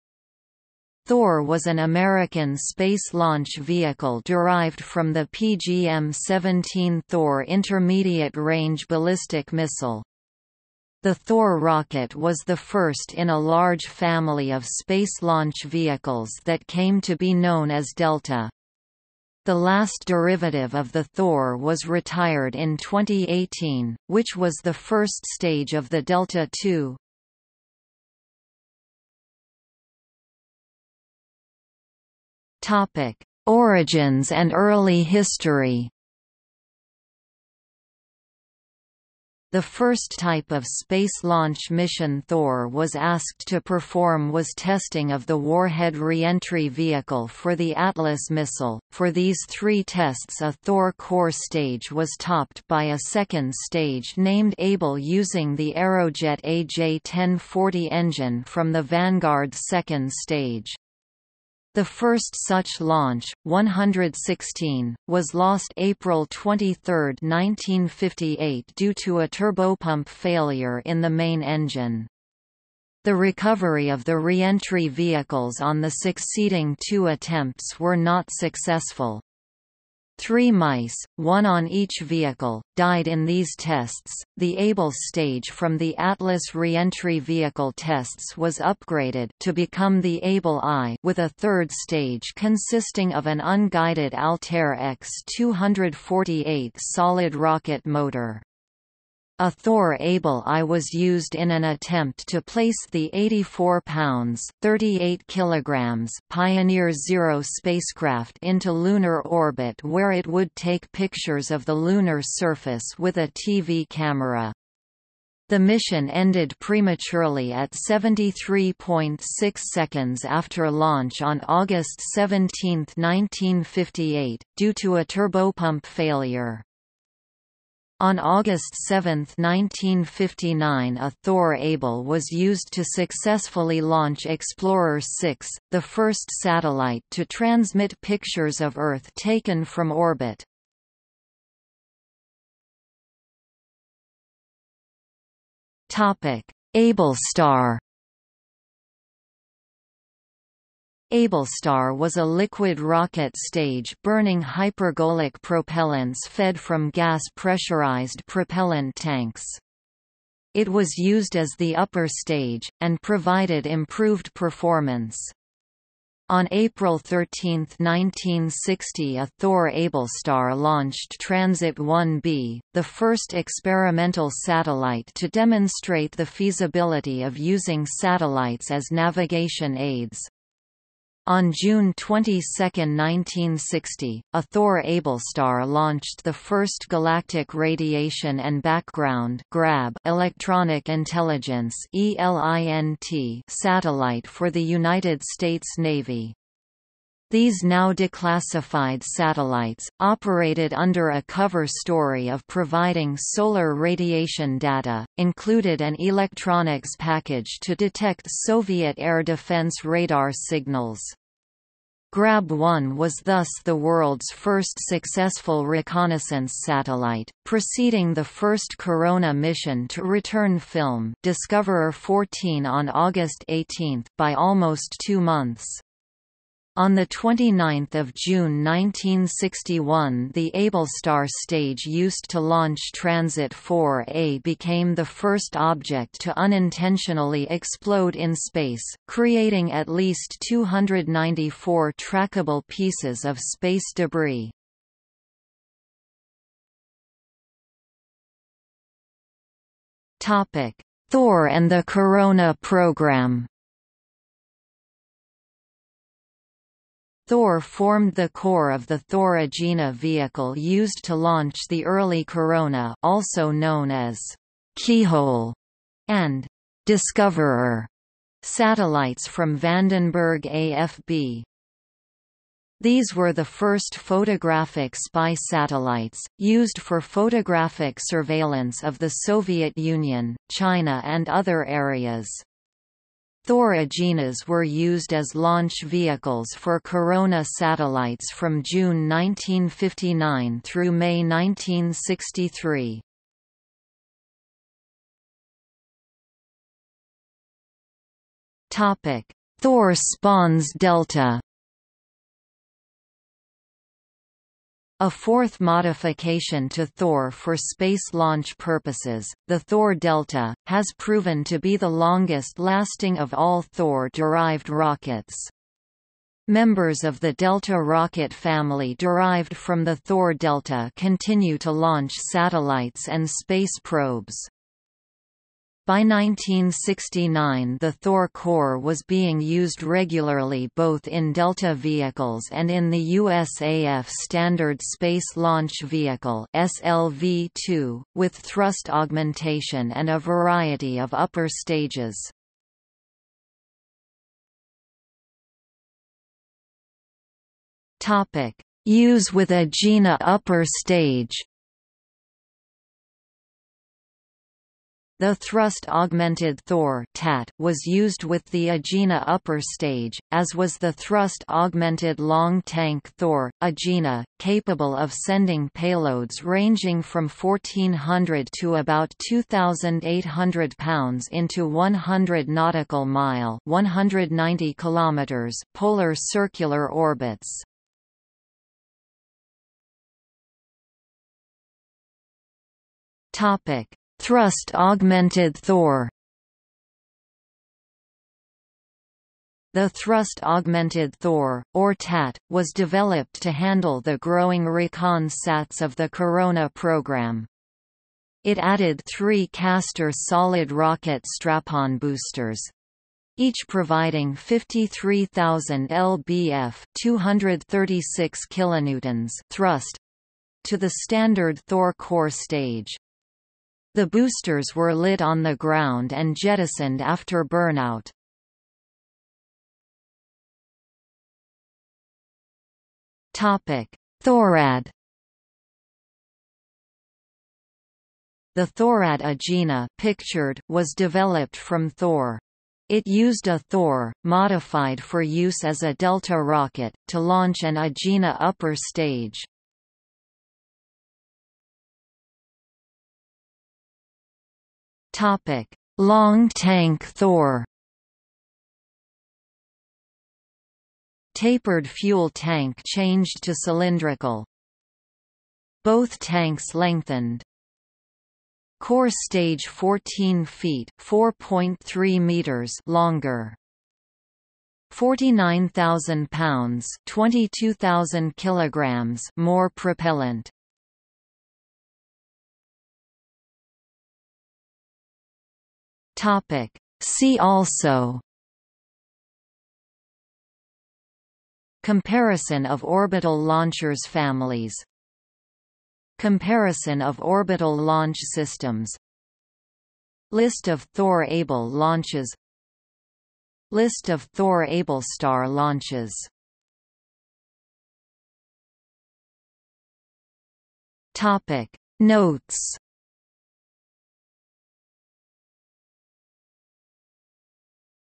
Thor (rocket family) | Wikipedia audio article
00:00:29 1 Origins and early history
00:02:22 1.1 Ablestar
00:04:06 2 Thor and the Corona program
00:04:55 3 Thor spawns Delta
00:05:44 4 Use with Agena upper stage
00:06:19 5 Thrust Augmented Thor
00:06:59 6 Thorad
00:07:21 7 Long Tank Thor
00:07:52 8 See also
00:08:17 9 Notes

- Socrates

SUMMARY
Thor was an American space launch vehicle derived from the PGM-17 Thor intermediate-range ballistic missile. The Thor rocket was the first in a large family of space launch vehicles that came to be known as Delta. The last derivative of the Thor was retired in 2018, which was the first stage of the Delta II.